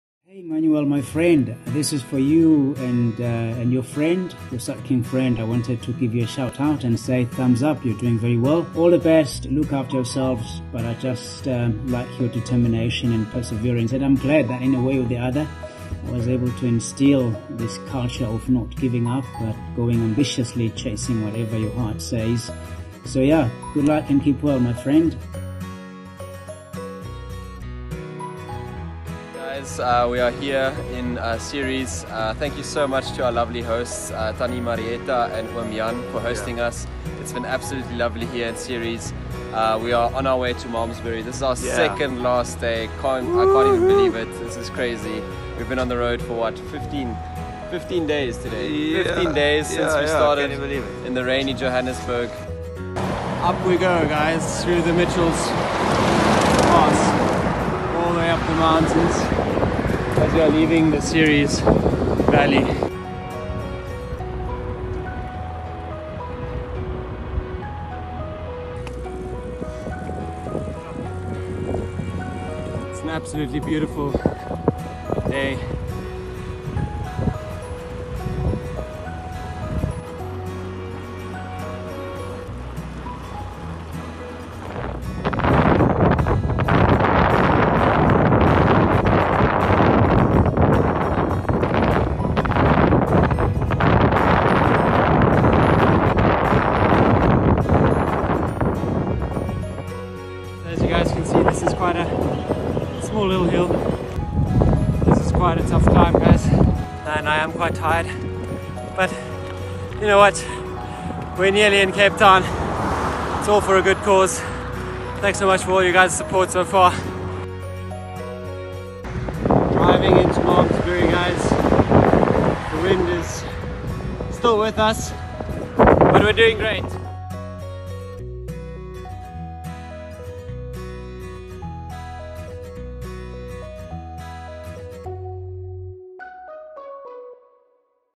Day 13 Ceres - Malmesbury
We are two ordinary guys cycling over 1600km in support of mental health.

Using nothing but our bicycles, some panniers and a whole lot of pedal power, our journey takes us from the bustling streets South of Johannesburg to the Free State, all the way down through the heart of the Karoo desert, into the luscious valley of Ceres and finally onto the shores of the Blouberg strand in Cape Town. Along the way we hope to elevate the conversation around mental health in South Africa and supporting SADAG at the forefront of this.